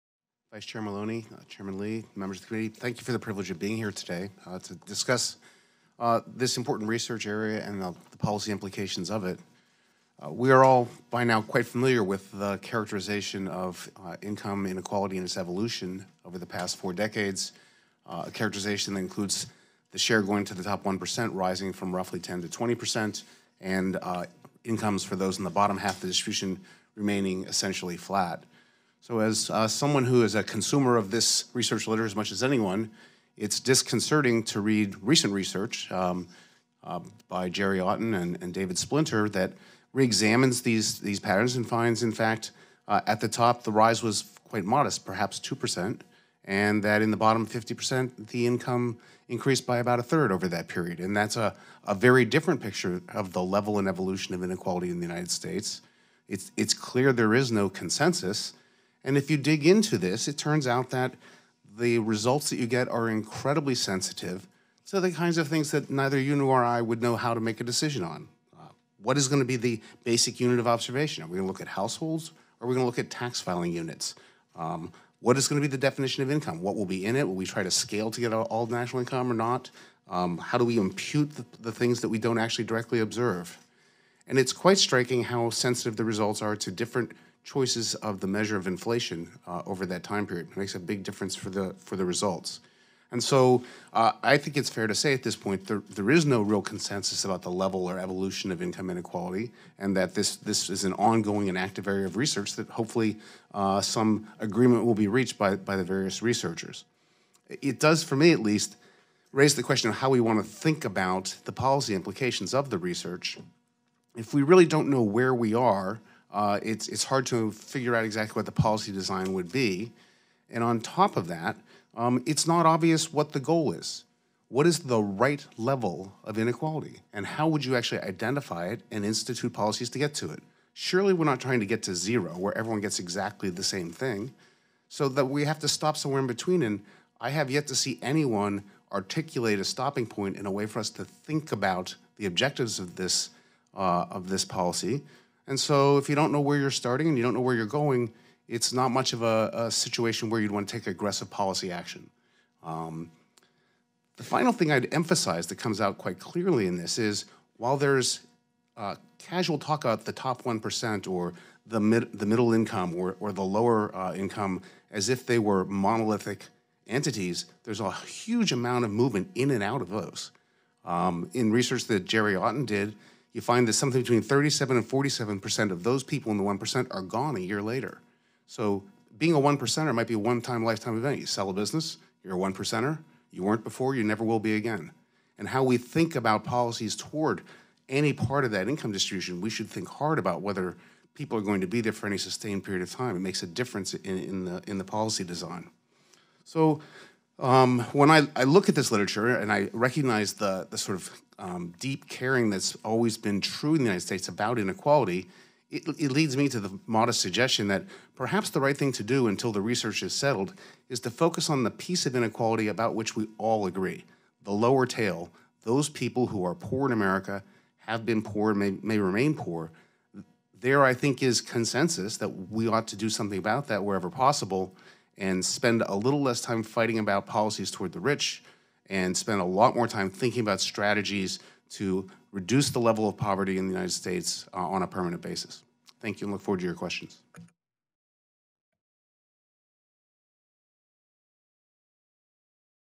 Dr. Douglas Holtz-Eakin Testimony Before the Joint Economic Committee, October 16, 2019
Dr. Douglas Holtz-Eakin, President of American Action Forum, offers testimony during the JEC hearing “Measuring Economic Inequality in the United States” on October 16, 2019.

Learn more about the important work of the JEC and The Social Capital Project